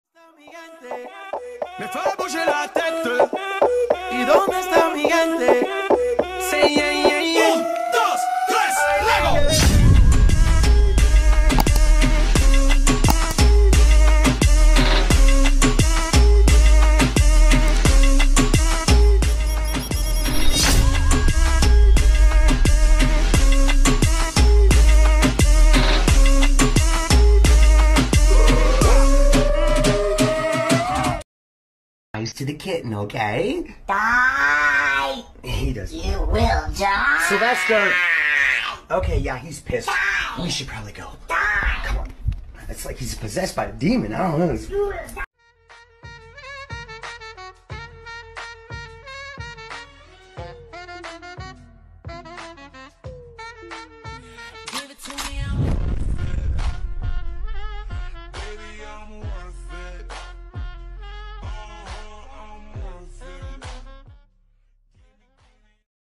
👿😈You Will Die!!!👿😈 Meme || Gacha Club || Original..? || AngieCandy_16
Hello my Angels!! I made a funny short clip/Meme for you guys today ft. My cat IRL!